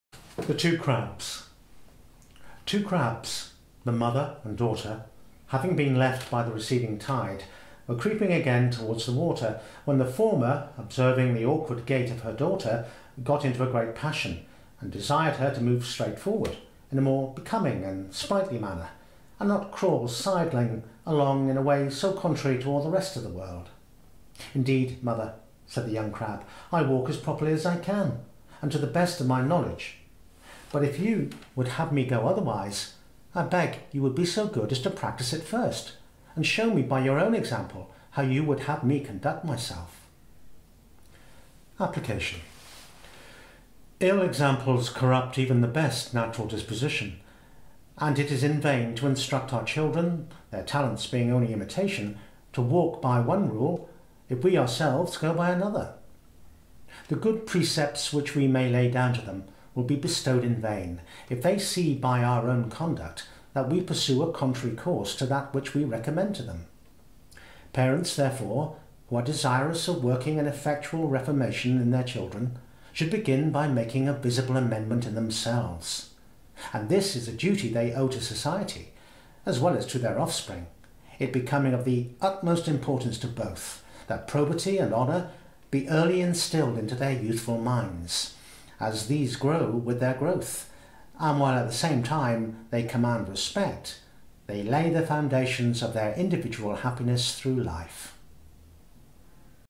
The Two Crabs
From the Fables of Aesop and others with designs on wood by Thomas Bewick. Published 1818 read out loud at Cherryburn 2018.